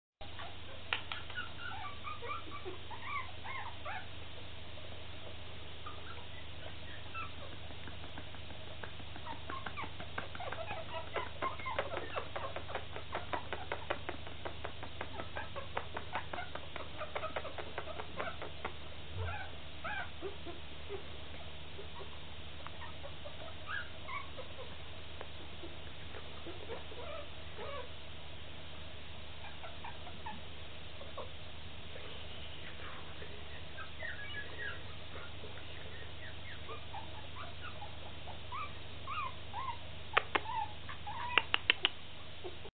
Bichon Bolognese Puppies & Christmas Tree
Uilaki's White Beauty® Bolognese Kennel / Canisa Bolognese, Ro

Puiuti bolognese 3 saptamani sub bradul de Craciun.